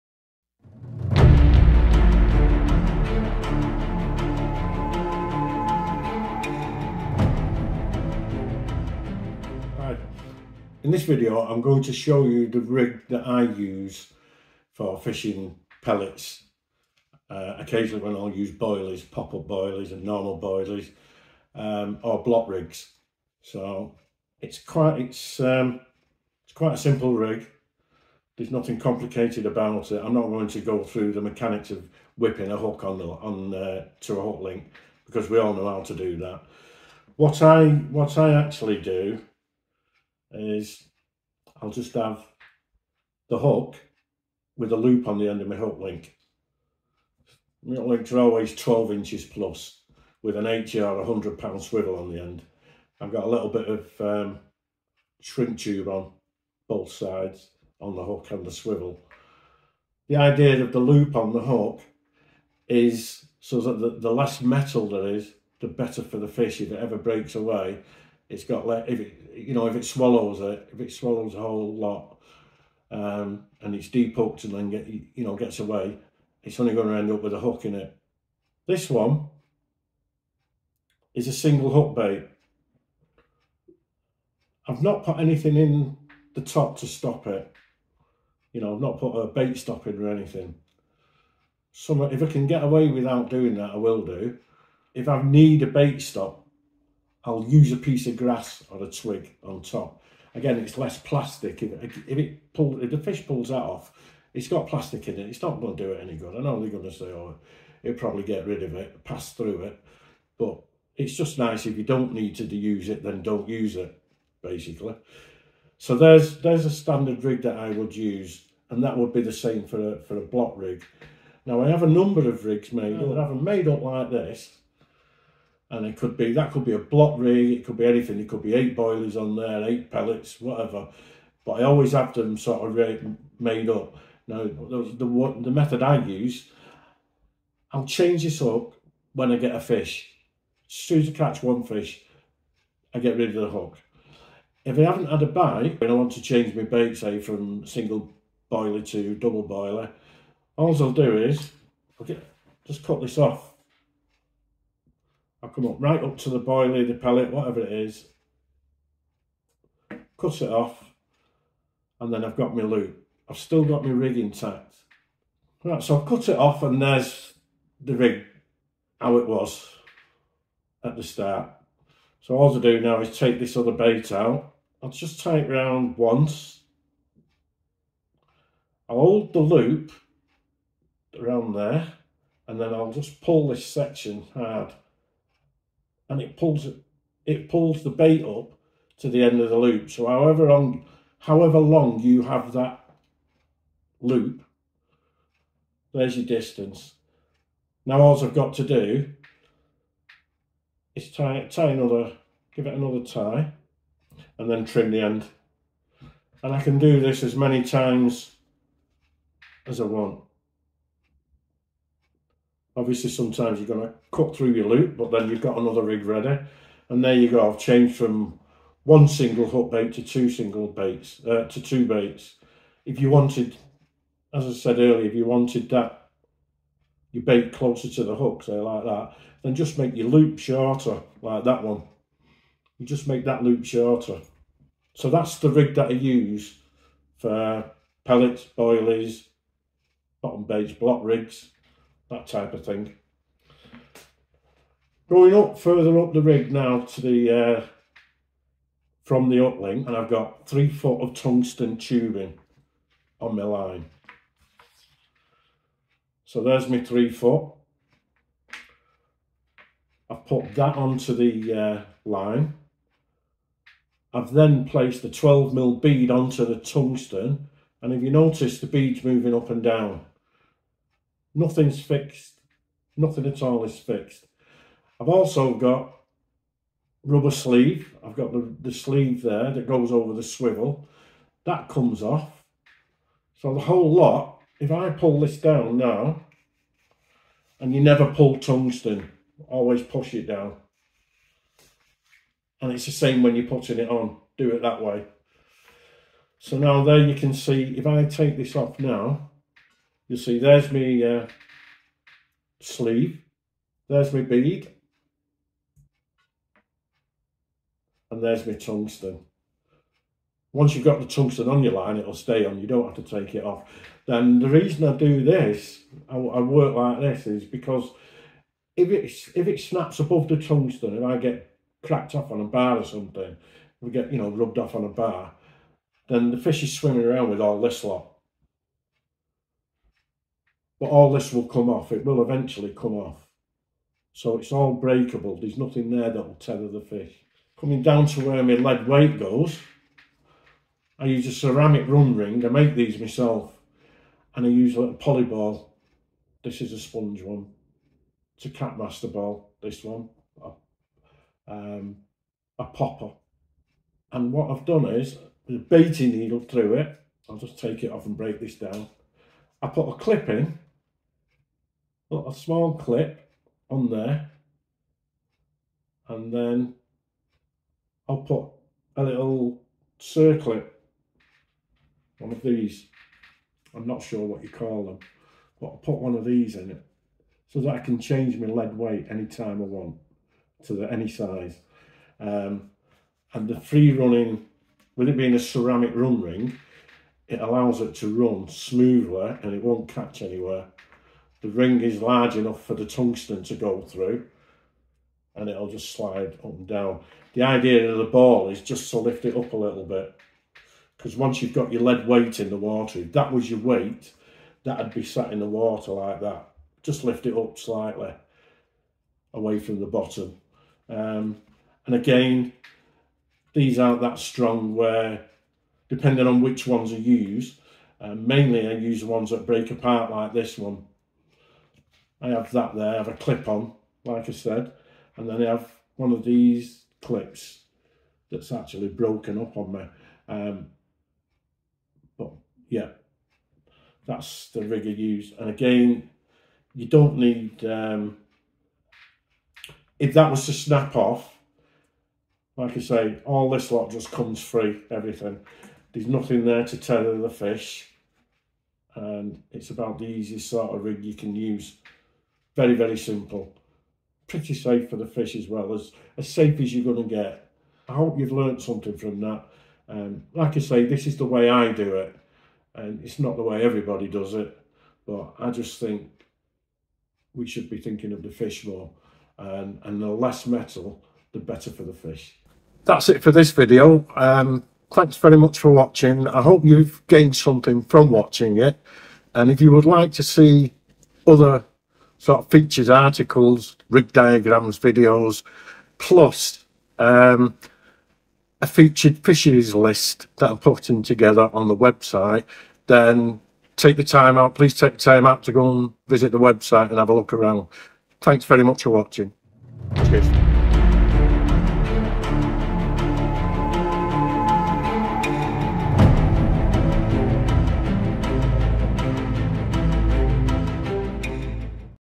My Basic Pellet Rig And Lead Setup
My basic rig for pellets and bollies complete with running lead setup.